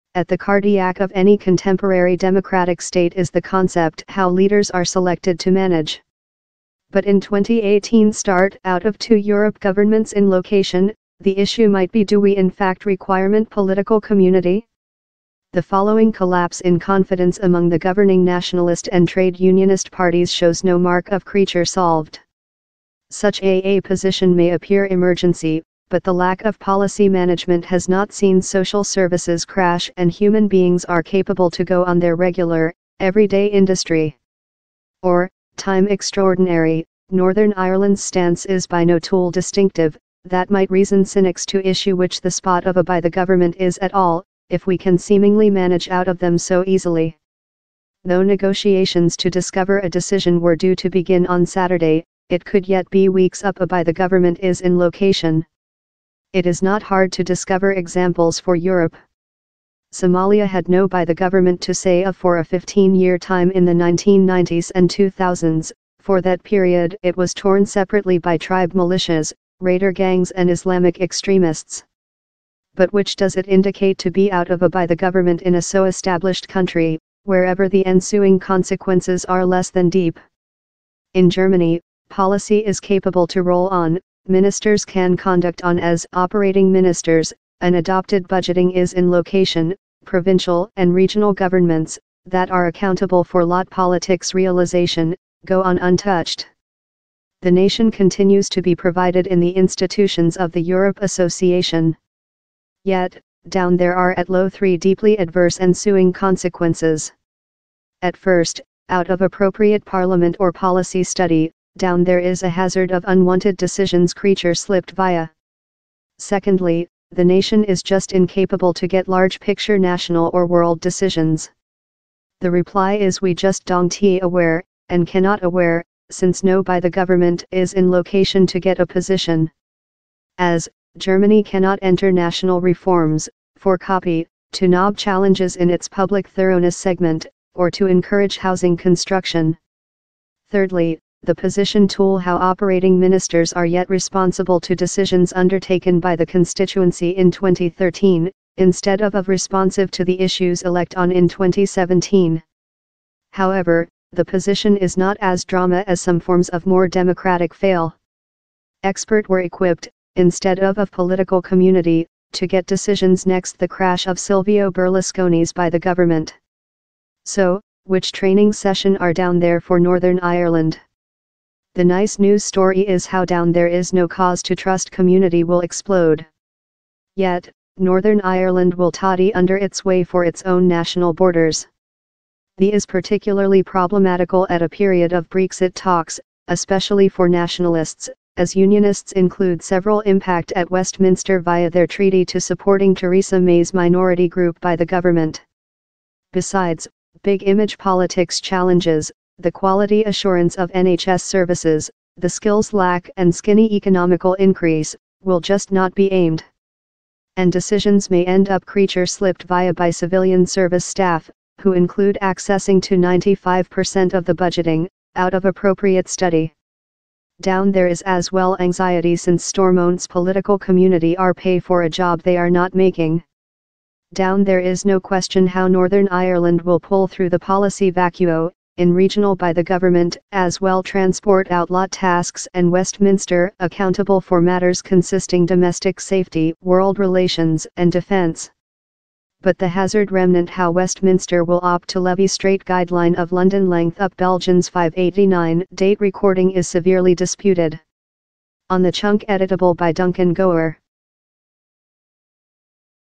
The Countries How Get By Out Of A By The Government
If countries are able to survive without elected leaders in place, do we actually need politicians?